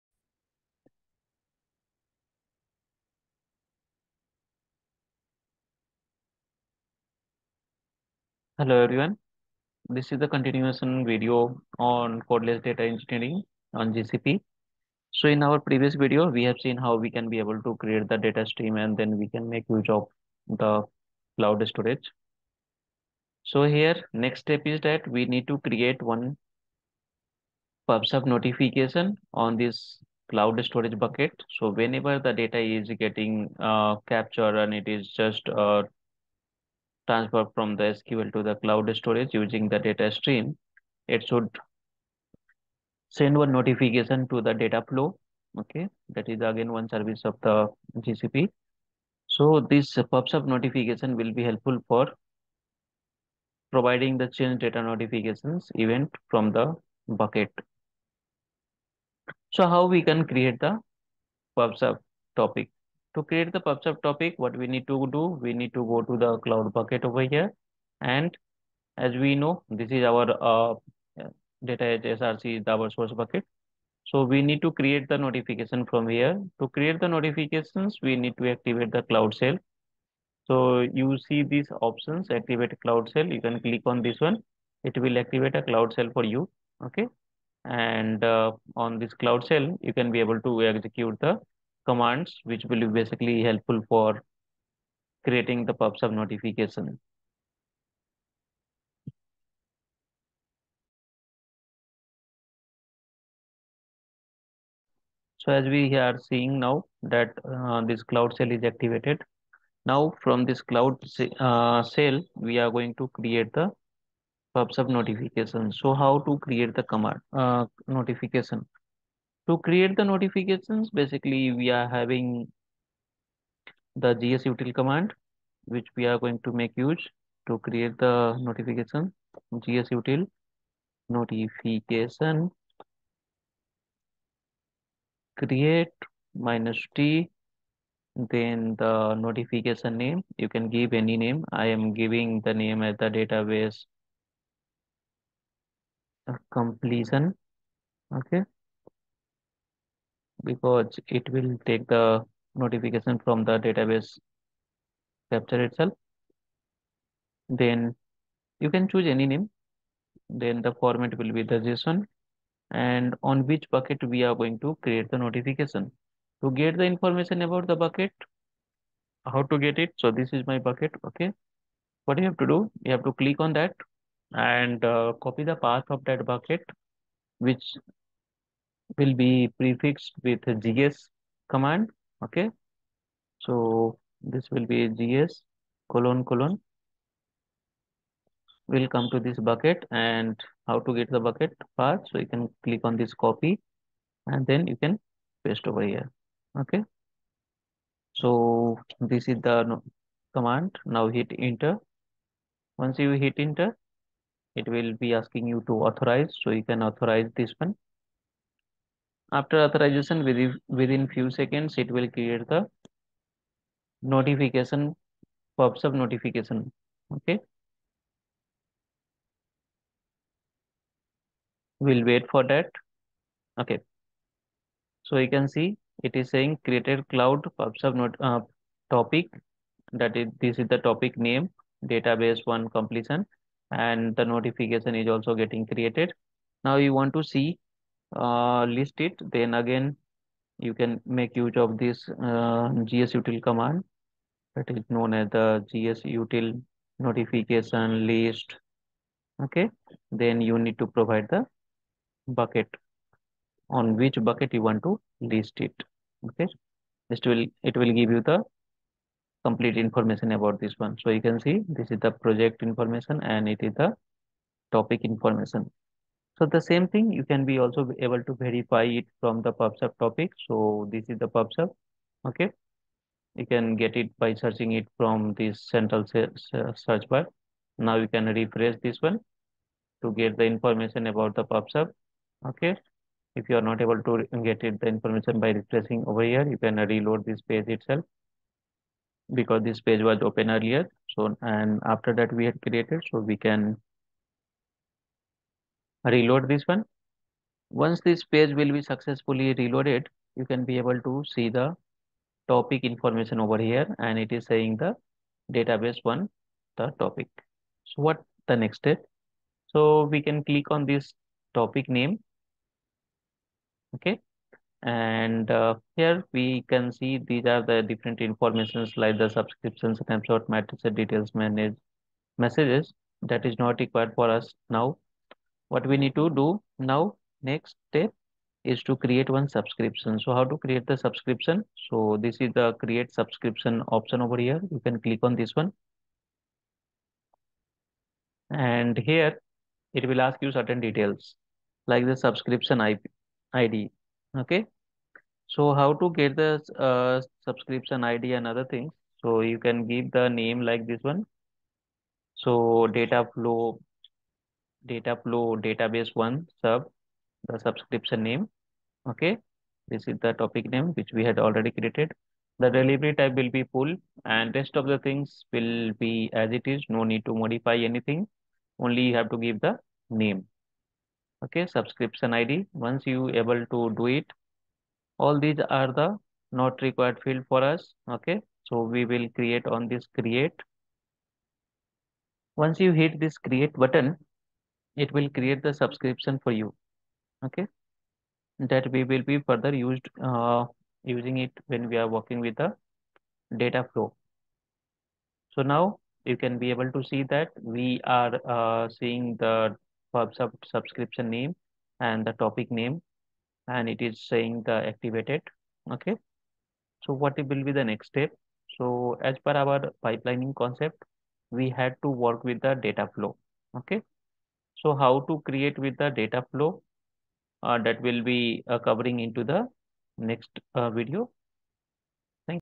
How to create pubsub topic and notification on gcp bucket || DataEdge Learning
To create a topic, follow these steps:
In the Google Cloud console, go to the Pub/Sub Topics page. Go to Topics.
Click Create topic.
In the Topic ID field, enter an ID for your topic.
Retain the option Add a default subscription.
Do not select the other options.
Click Create topic.